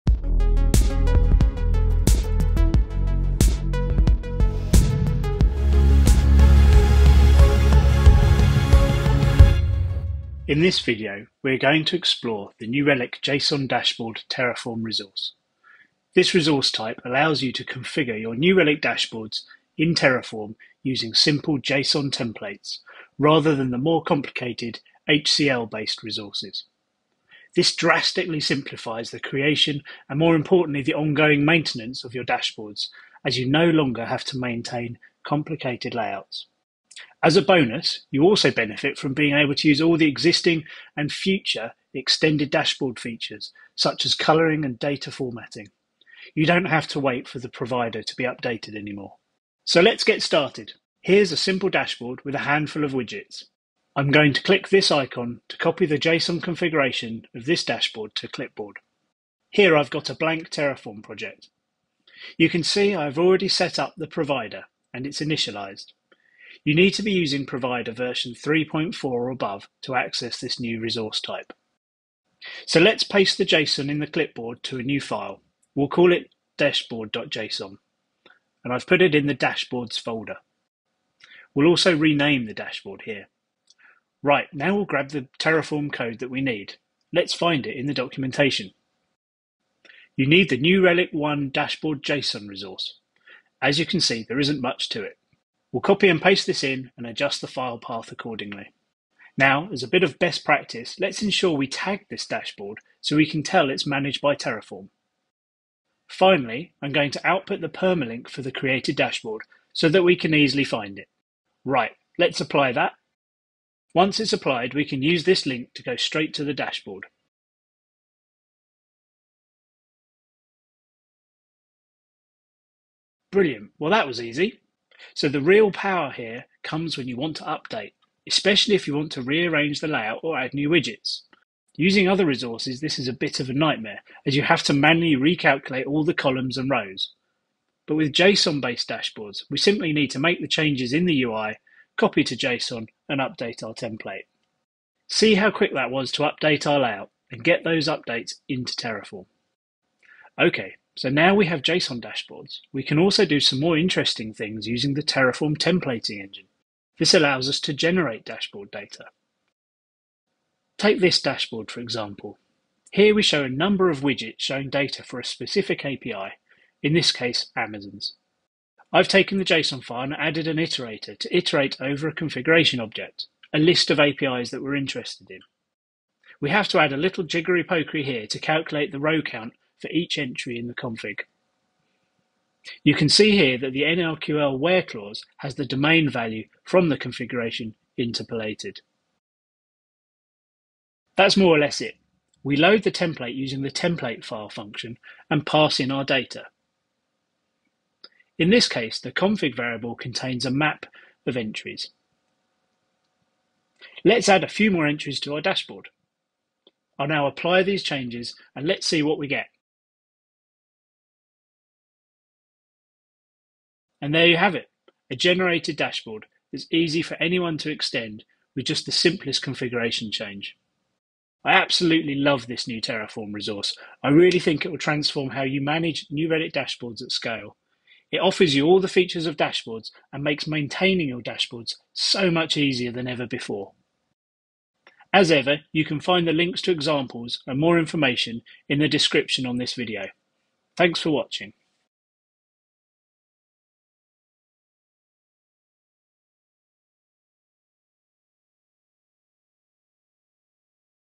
Easy dashboard maintenance in Terraform
Quick and easy dashboard maintenance in Terraform using the JSON dashboard resource.

Reduce the toil of managing your dashboards in Terraform by using the JSON dashboard resource. In this episode James shows how to get a dashboard up and running and shows how you can easily update and maintain the dashboards. He also demonstrates how you can generate the complex content dashboards from simple user input.

Presenter: James Buchanan; Product: Dashboards